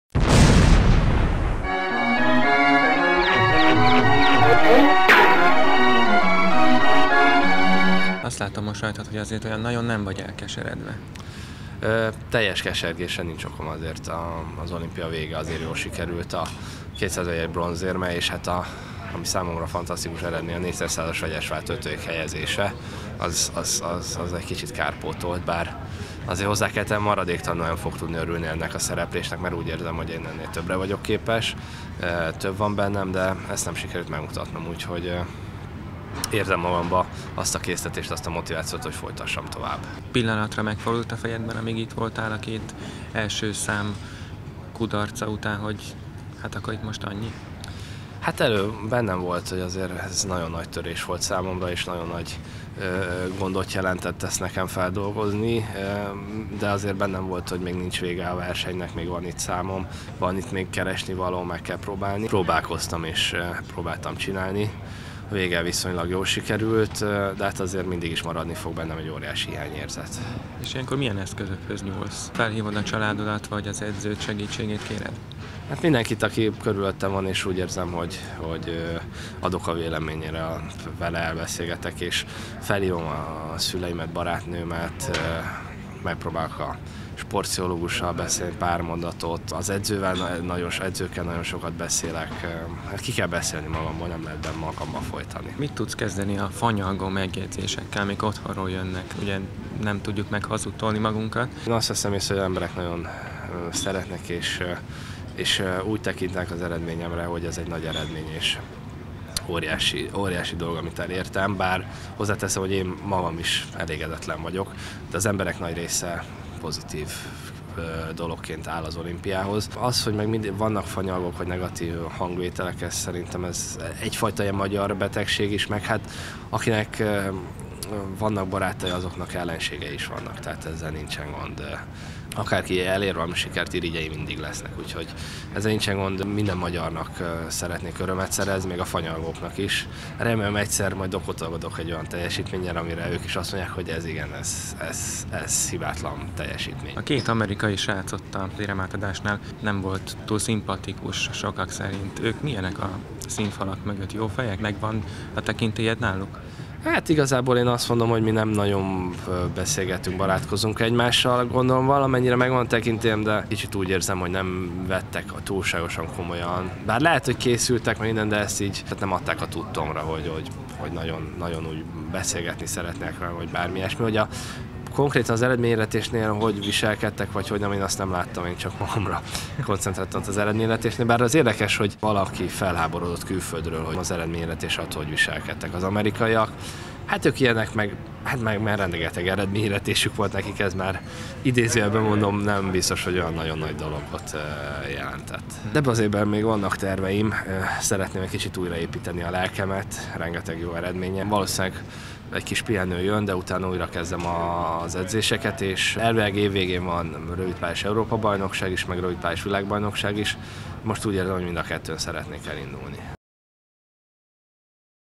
Cseh Laci: Újra szeretném építeni a lelkemet
A 200 méteres vegyesúszásban olimpiai bronzérmet szerző magyar úszó nincs maximálisan elkeseredve a londoni szereplést illetően. Úgy érzi, lesz motivációja a folytatásra, azzal pedig nincs gond, hogy akadnak fanyalgók.